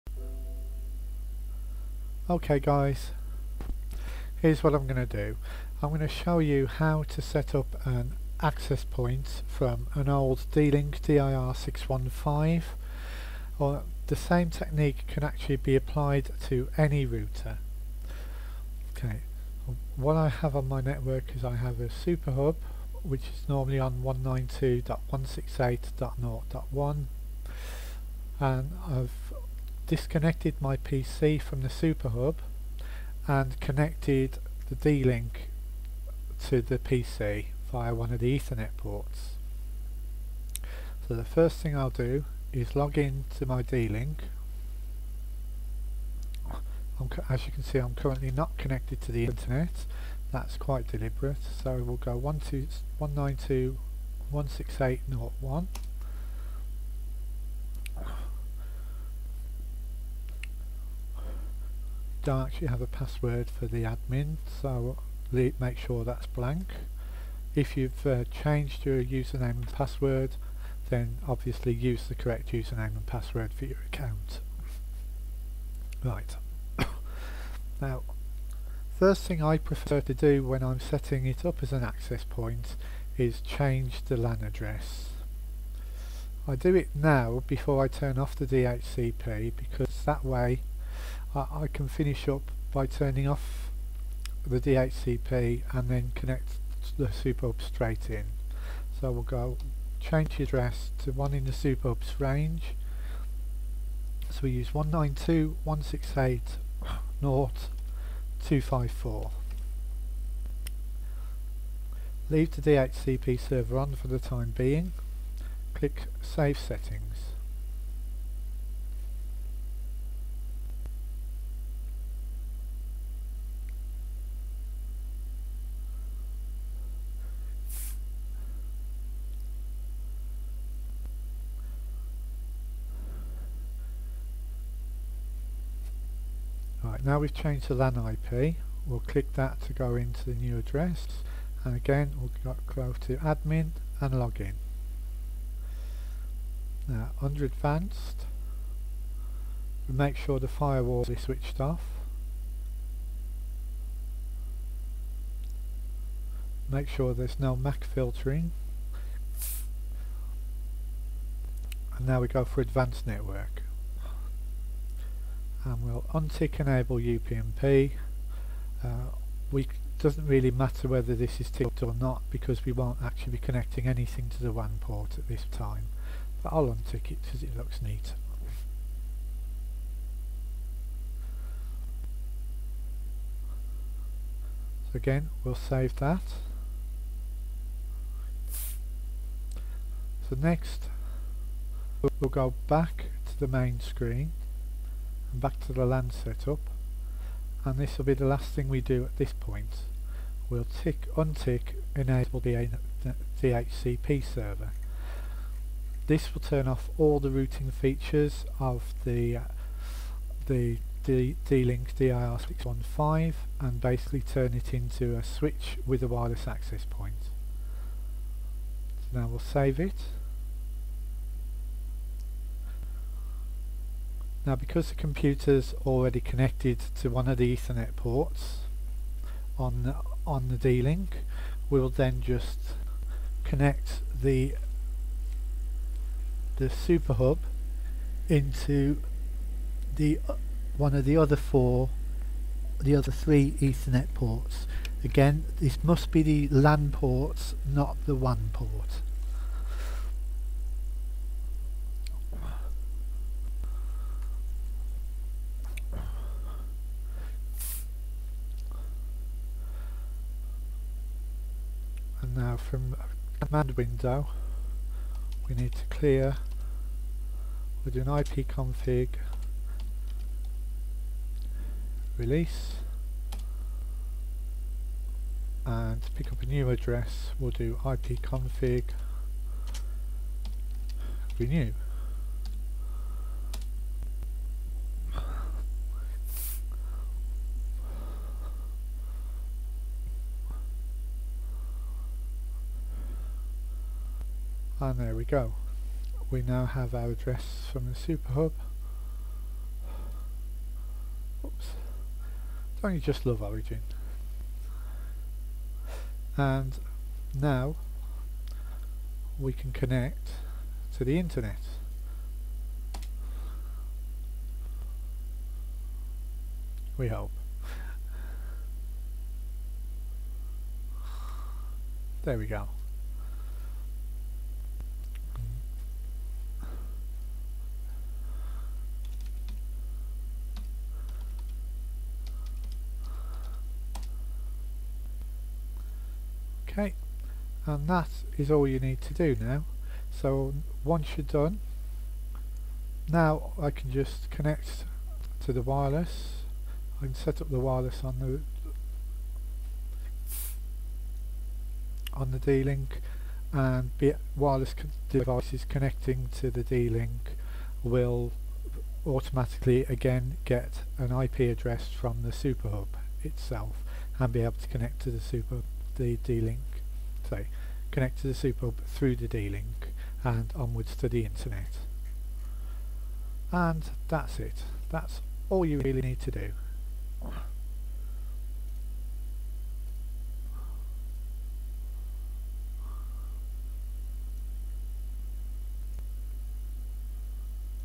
Configuring router as an access point
Configuring a Dlink Dir615 as an Wireless access point, connected to a main router